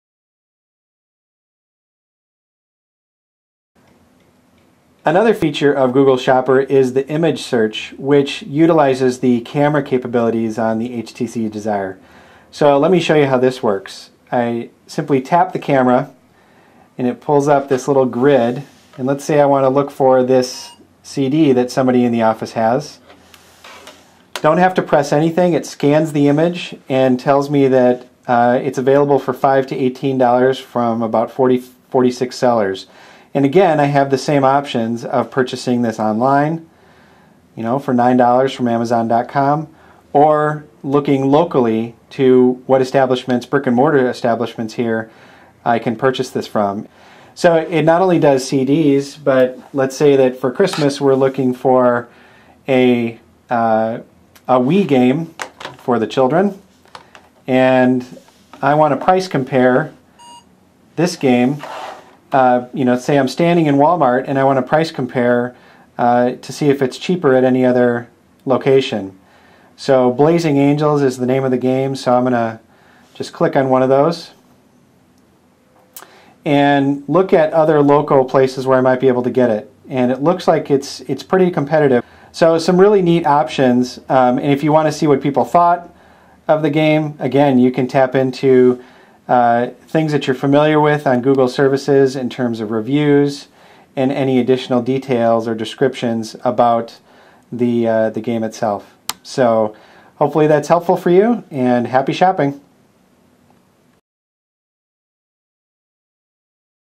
Android App: Google Shopper - Image Search
This video shows you how to do an image search using the Google Shopper App on your Android device.

Google Shopper is available on the Android Market and allows you to take pictures of books, movies, video games and more, speak your search or  scan their barcode to compare prices, find local and online retailers carrying the product and even get directions to the location closest to you.